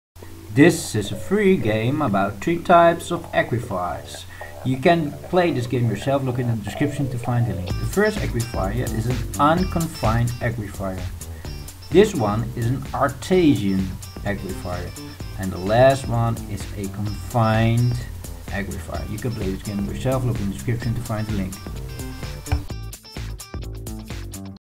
3 typer av akviferer, trånga, unconfined och artesiska sandskikt stängs med salt eller lera
3 typer av akviferer, trånga, unconfined och artesiska sandskikt stängs med salt eller lera